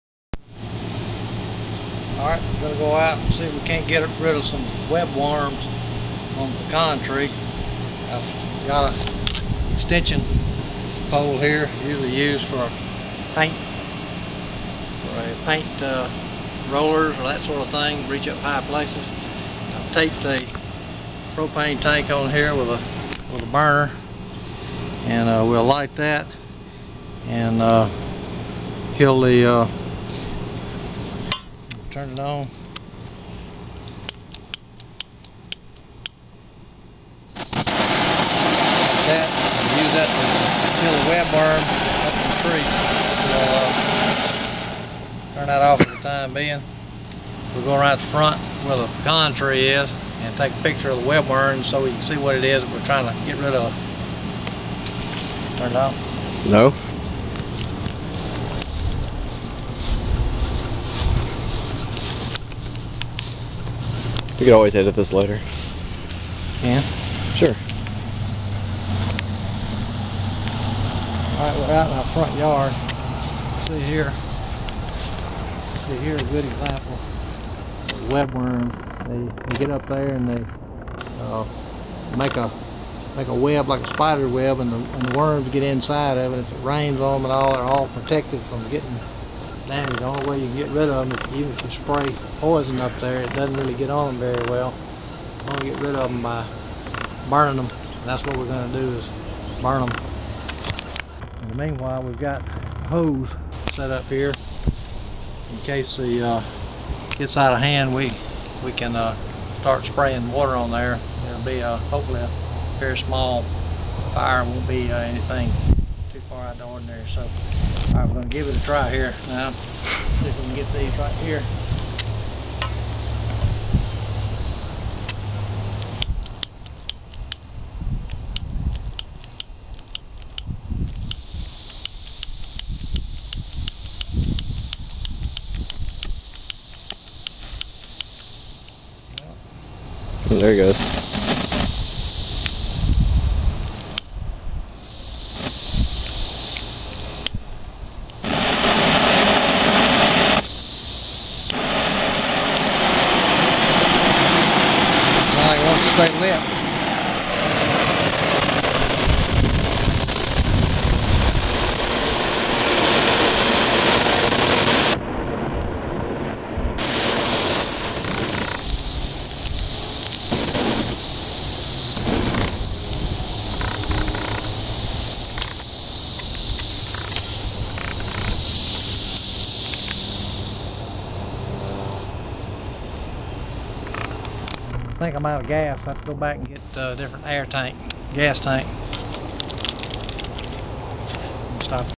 How to Kill Web Worms Part 1
A tutorial of how to kill Web Worms by Berry Cannon IV. Filmed by son, Berry Cannon V. This is Part 1.